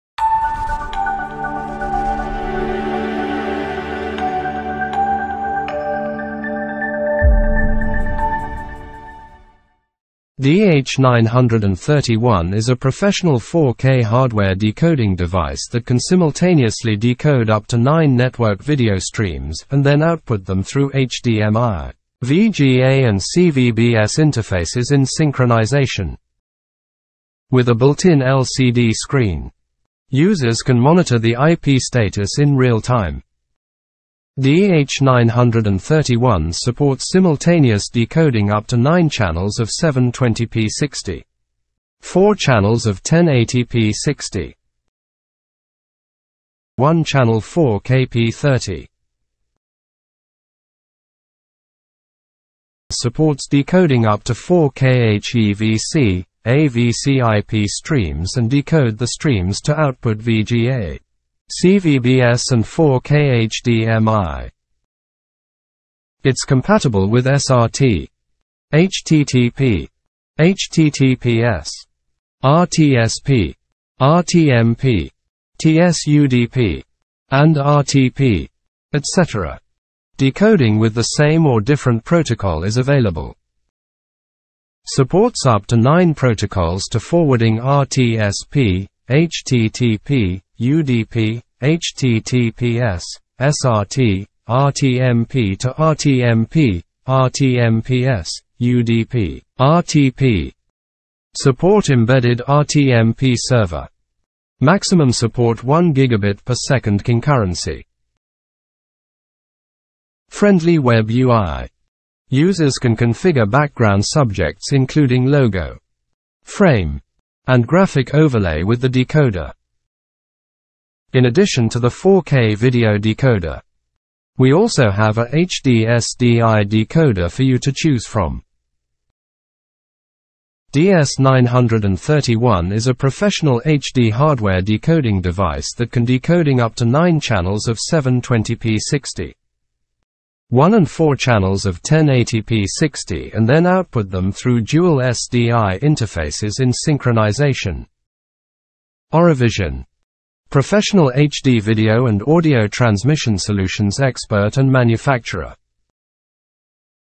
ORIVISION Multi-channel Video Decoder
ORIVISION DH931 video decoder is a professional 4K H.265 (HEVC) hardware decoder that brings multiple video streams to HDMI/CVBS/VGA. Without relying on a computer, it decodes any IP streams including HTTP/SRT/RTSP/RTMP/UDP/RTP/UTP, etc, and outputs the video to the display wall in up to 9 grids or free layout, and dynamic switching is supported.

It has high integration and low costs and can be widely used in various HD video systems. It supports POE power supply, LCD real-time display to ensure uninterrupted video streaming. And also supports an RTMP server for direct connection with video encoder.

Products Features:
• Support H.265 and H.264 decoding
• Support 1XHDMI,1X VGA,1XCVBS and 1X audio output
• Support embedded RTMP server, maximum support 1Gbps concurrency
• Support HTTP/HTTPS/RTSP/RTMP/SRT/UDP/RTP protocol
• Support 9channels RTSP/HTTP/UDP/HTTPS/SRT/RTMP forward to RTMP/
RTMPS/UDP/RTP
• LCD display device name, IP address, resolution and working status
• Support up to 4K (3840×2160@60hz/30hz) resolution output
• Support to set the cache
• Support 1ch, 4ch, 9ch decoding simultaneously
• Support boot screen customization
• MJPEG/JPEG Baseline decoder
• MPEG1 Audio Layer 2
• 1000M network interface uses full duplex mode
• Support POE power supply(optional)
• Support SDK, secondary development